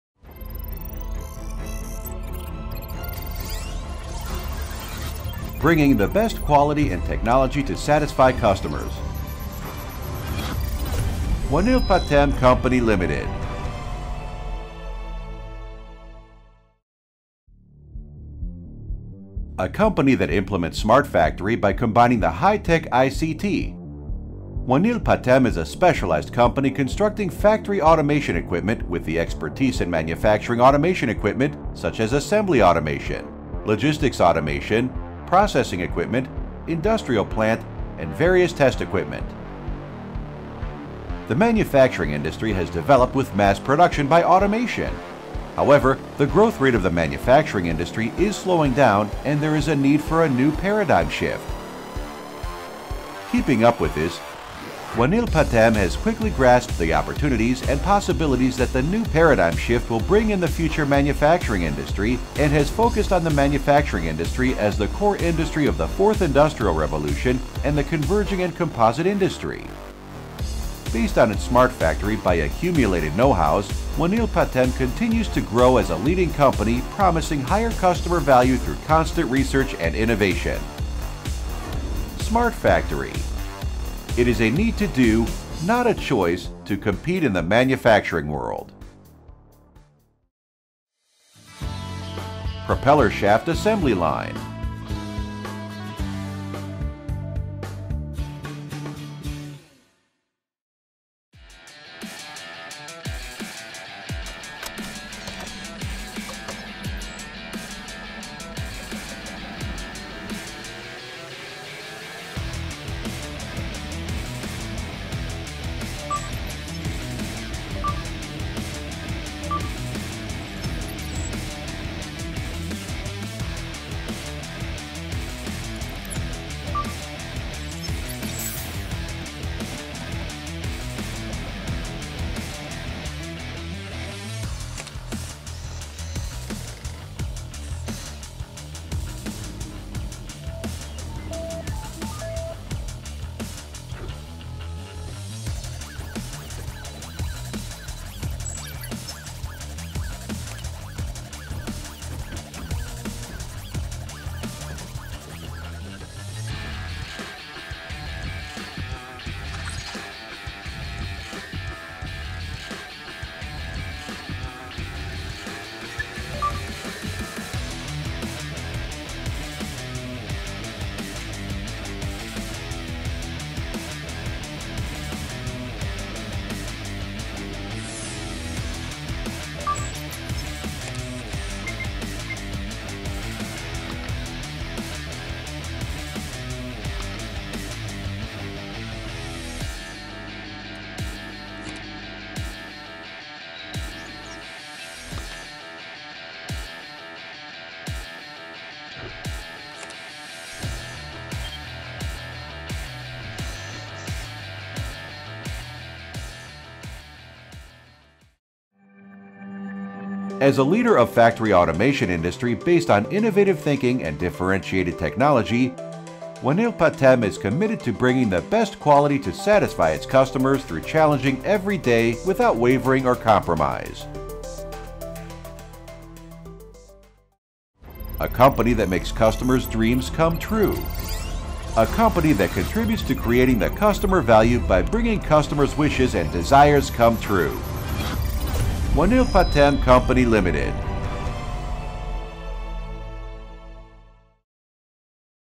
ALL 자동! 공장자동화 전문 메이커! - 원일파템
원일파템은 공장 자동화 기계 설비 제조 및 생산업체이며, 그 외에도 정밀측정기기, 로봇 등 산업처리공정 제어장비를 제조하고 있습니다.

Wonil fatem is a manufacturer of factory automation machinery equipment, and also manufactures industrial process control equipment such as precision measuring equipment and robots.

▶ 생산제품/Products
- Assembly Automation / Inspection and Test Equipments / Private Processing Machines
- 조립자동화 / 각종시험장비 / 가공전용설비
대구경북기계협동조합 해외마케팅
대구시 기계산업 수출역량 강화사업
Daegu Gyeongbuk Machinery Cooperative, DGMC